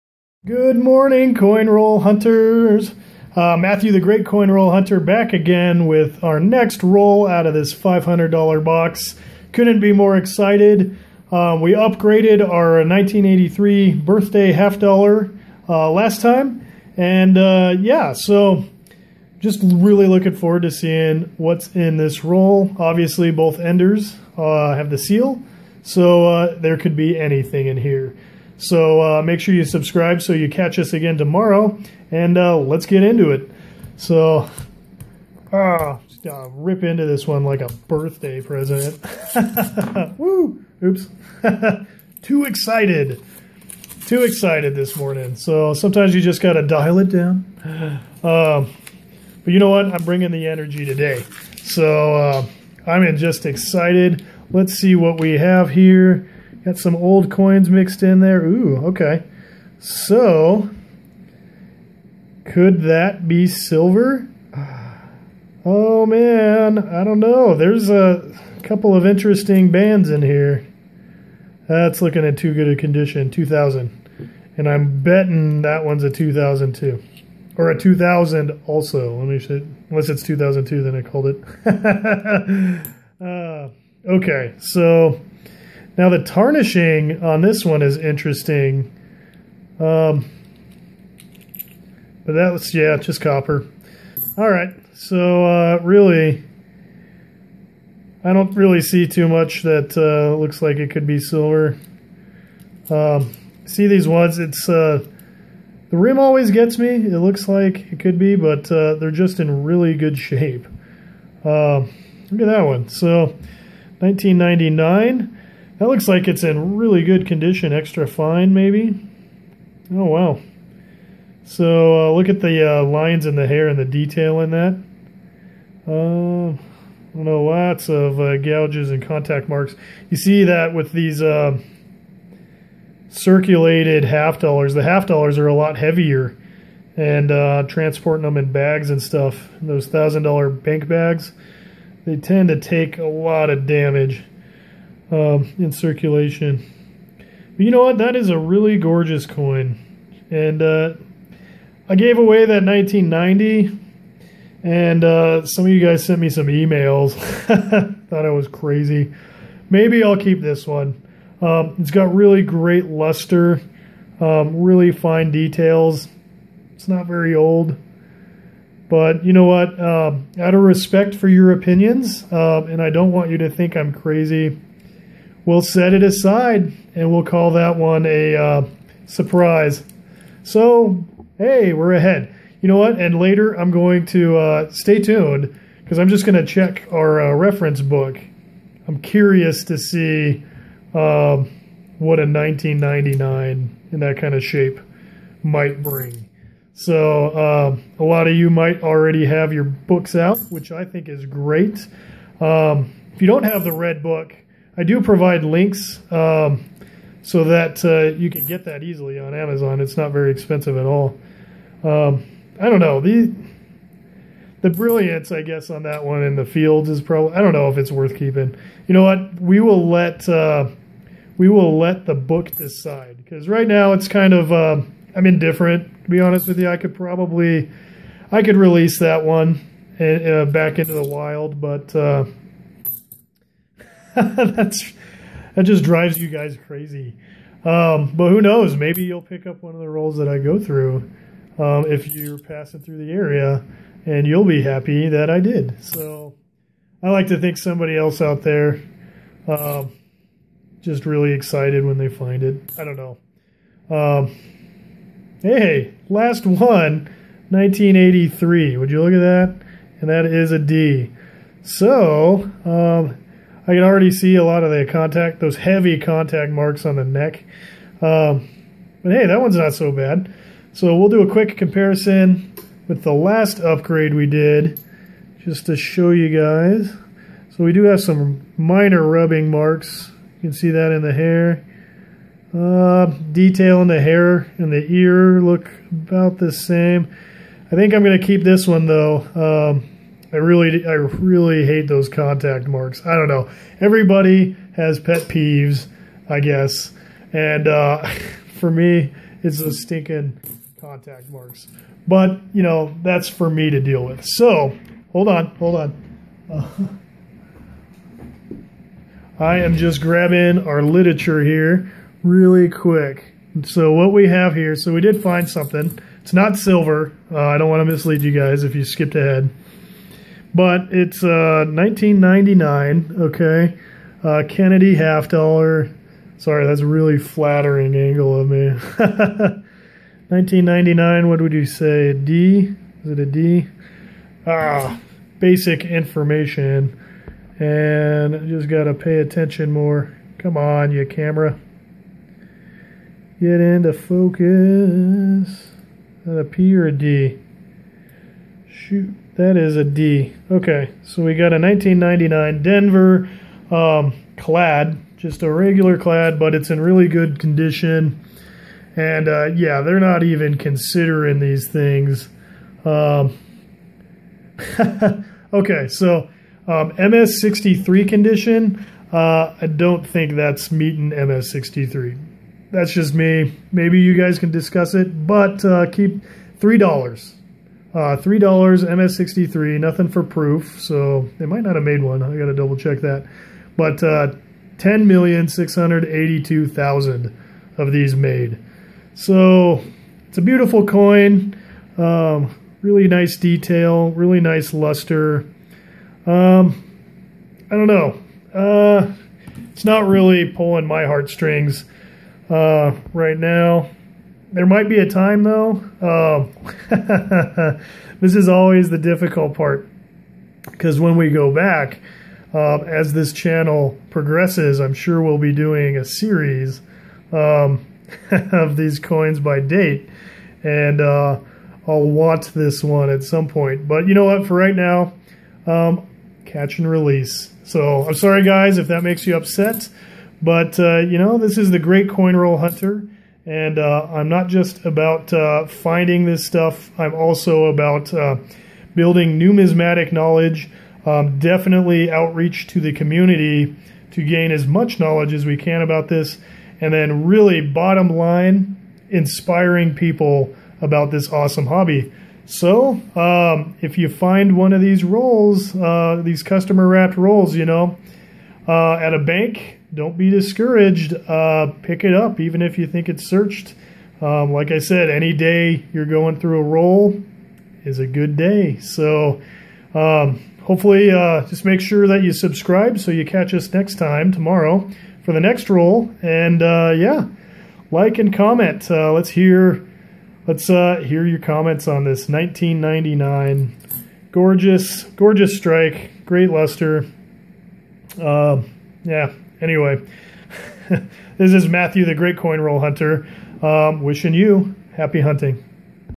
ROLL 20 COIN ROLL HUNTING SILVER HALF DOLLARS
Coin roll hunting for silver. 20th roll in a box of half dollars.
Share your coins with a USB microscope

Get the book before the coin:
Guide Book of Half Dollars